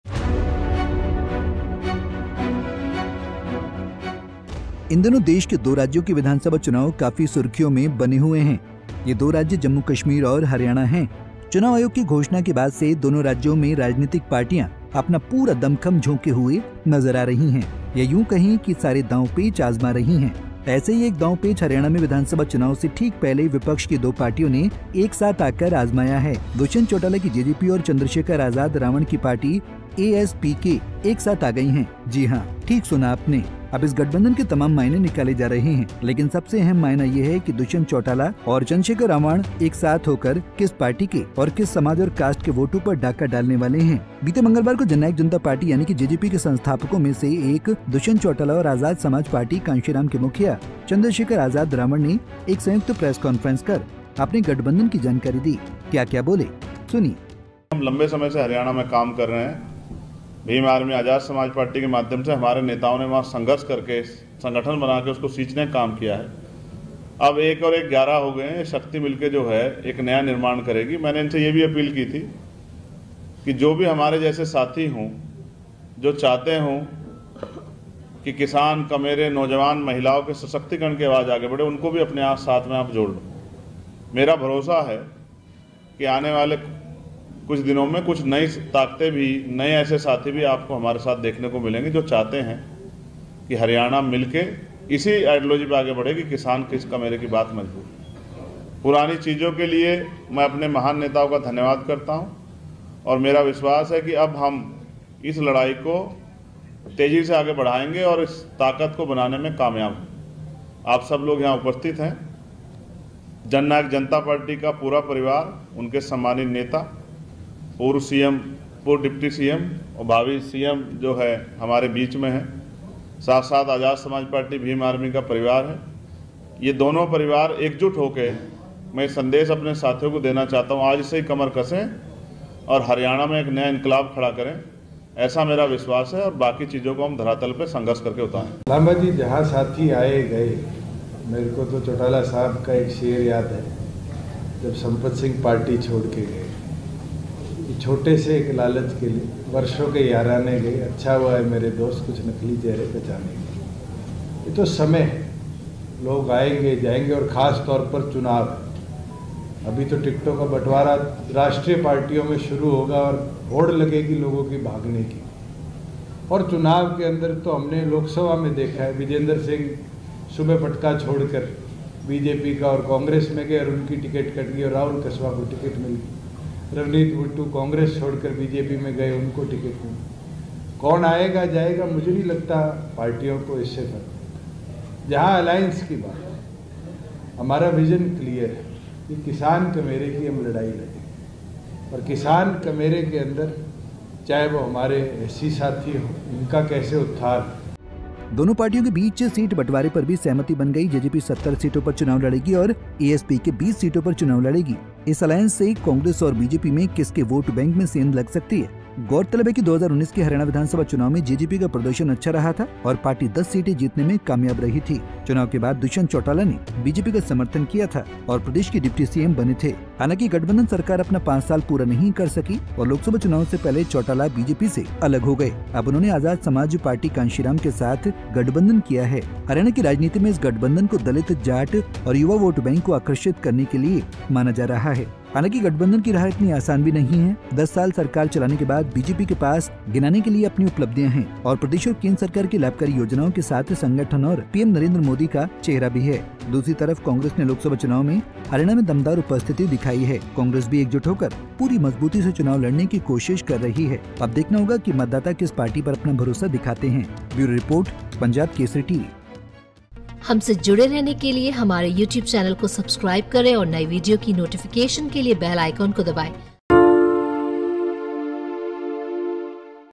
Haryana Assembly Election 2024: Dushyant Chautala के साथ आए Chandrashekhar Azad Ravan, अब होगा खेल?
इन दिनों देश के दो राज्यों के विधानसभा चुनाव काफी सुर्खियों में बने हुए हैं... ये दो राज्य, जम्मू-कश्मीर और हरियाणा हैं... चुनाव आयोग की घोषणा के बाद से दोनों राज्यों में राजनीतिक पार्टियां अपना पूरा दमखम झोंके हुए नजर आ रही हैं या यूं कहें कि, सारे दांव-पेंच आजमा रही हैं...